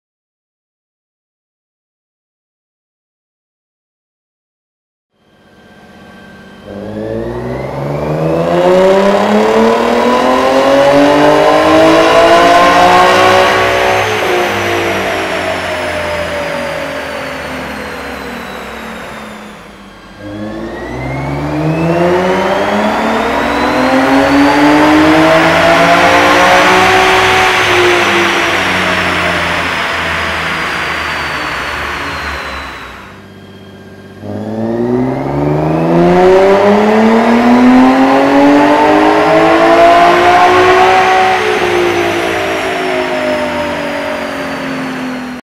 Nissan 370Z | Motordyne Engineering | UpRev Pro-Tune | Real Auto dynamics
2013 Nissan 370Z with the Amuse kit.
Motordyne ART Pipes
Motordyne E370 Exhaust
Stillen Gen3 Intakes

UpRev Pro-Tune